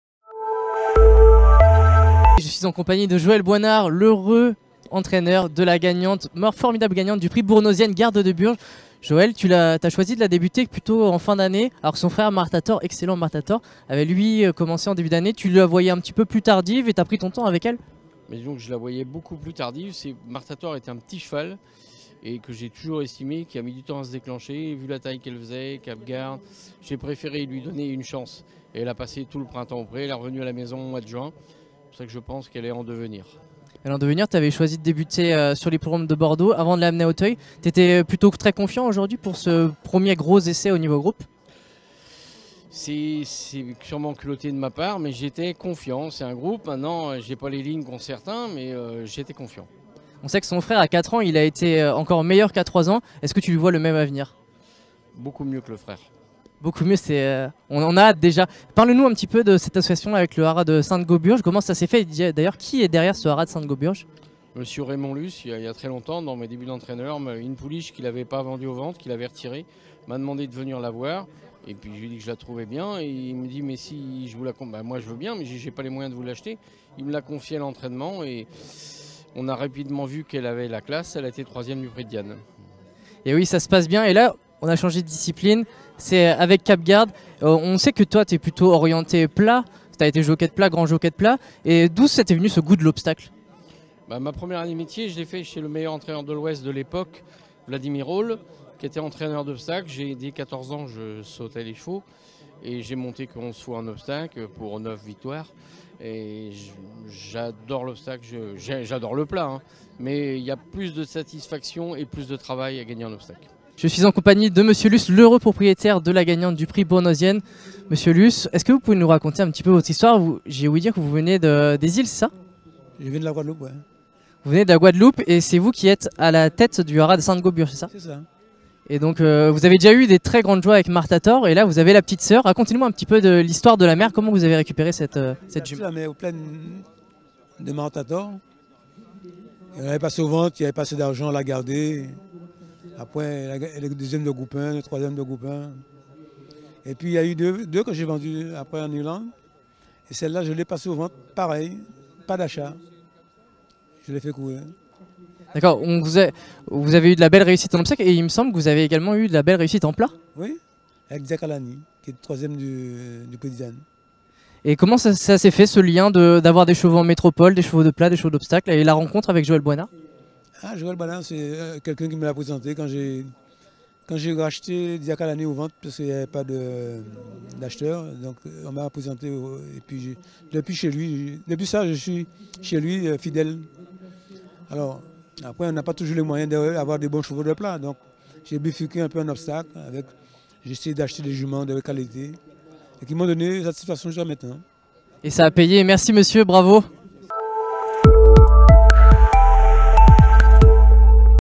Prix Bournosienne (Gr.2) 2022: le sacre de Garde de Burge, vu par Joël Boisnard et Raymond Luce
Respectivement entraîneur et éleveur de la pouliche, Joël Boisnard et Raymond Luce (Haras de Sainte-Gauburge), reviennent à chaud sur le sacre de Garde de Burge dans le Haras d'Etreham - Prix Magalen Bryant (Prix Bournosienne (Gr.2)), couru ce samedi 12 novembre, sur l'hippodrome d'Auteuil